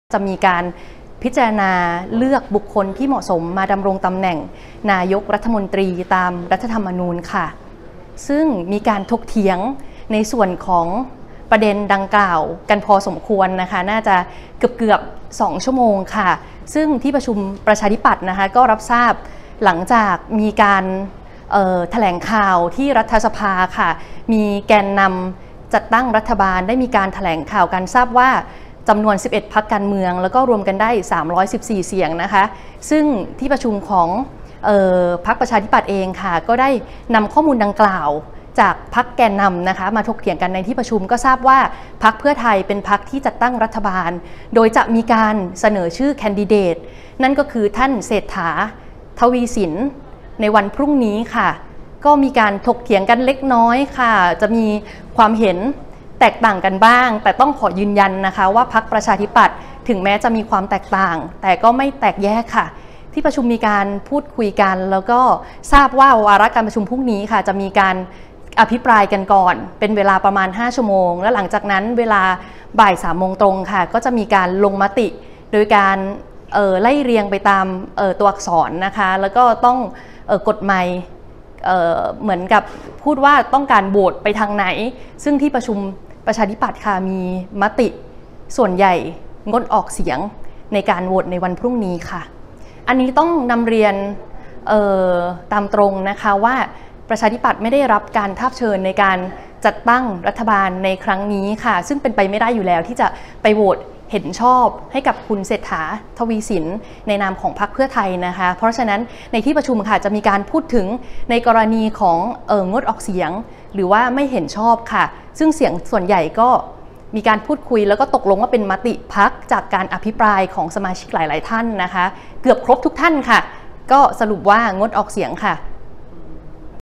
มติ ปชป.งดออกเสียงโหวต "เศรษฐา" หลังชวดร่วม รบ. ผู้ใหญ่พรรคขอไม่เห็นชอบเหตุไม่เคลียร์สังคม
ปชป.มีมติงดออกเสียงโหวต "เศรษฐา" นั่งนายกฯ หลังตกขบวนตั้งรบ. เผย "ชวน -บัญญัติ-จุรินทร์ -สรรเพชญ" ขอโหวตไม่เห็นชอบ อ้างไม่เคลียร์ข้อกล่าวหาสังคม-เทียบเคียง"พิธา"ไม่ได้